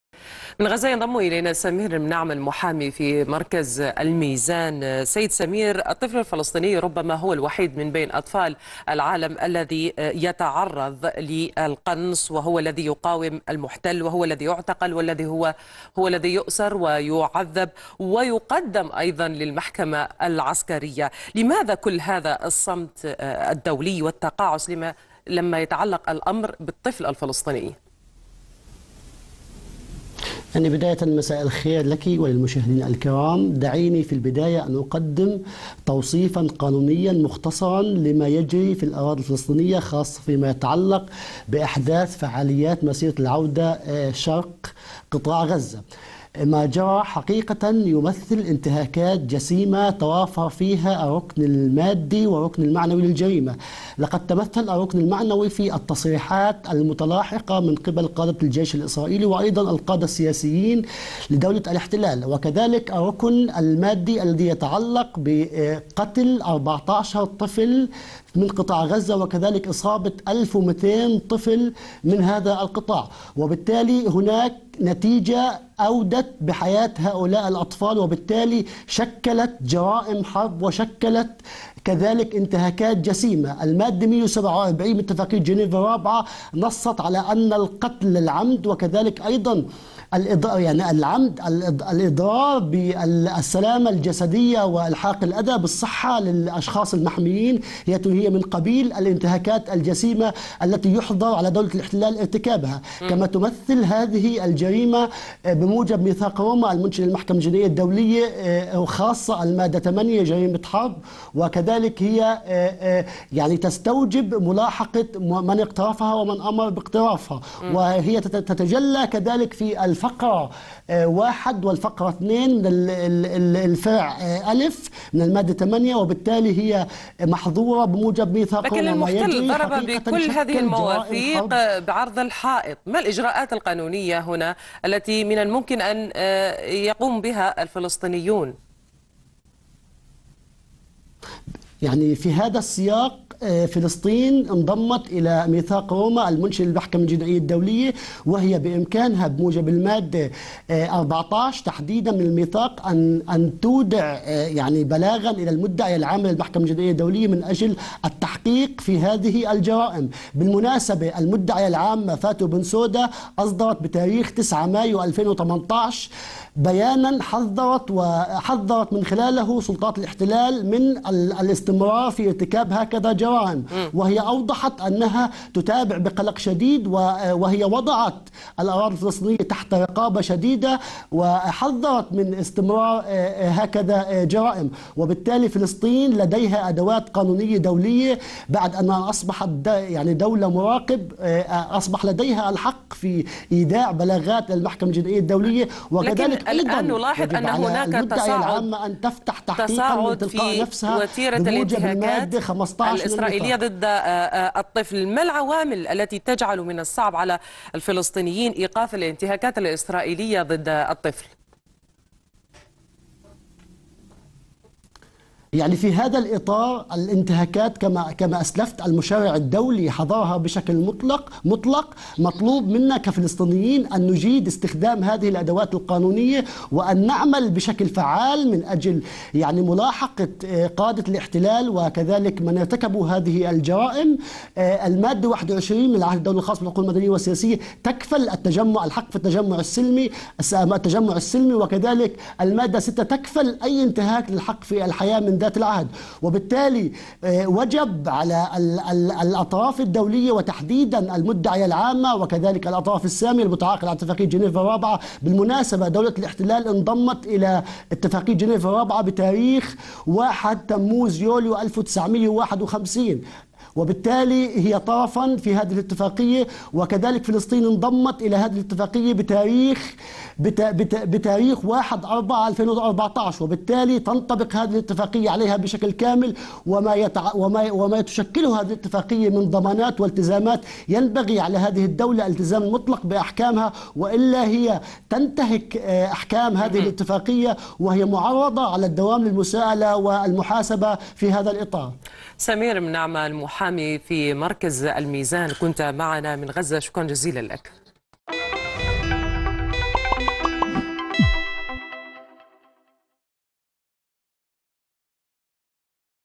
محام في مركز الميزان لحقوق الإنسان: استهداف الأطفال خلال مسيرات العودة انتهاك جسيم للقانون الدولي
يتعمد الاحتلال الإسرائيلي استهداف الأطفال الفلسطينيين بالقتل والإصابة والاعتقال، ومؤخرا تصاعدت وتيرة الهجمات والاعتقالات بحق الأطفال في محاولة من الاحتلال لكسر إرادتهم والنيل من عزيمتهم، الطفل عبد الرحمن نوفل أصابه الاحتلال بطلق متفجر في قدمه أثناء لهوه مع أصدقائه بالقرب من مسيرات العودة الكبرى، مما أدى إلى بتر قدمه المزيد من التفاصيل يتحدث سمير المناعمة، محام في مركز الميزان لحقوق الإنسان، مع سهام عبد القادر، في قناة الغد.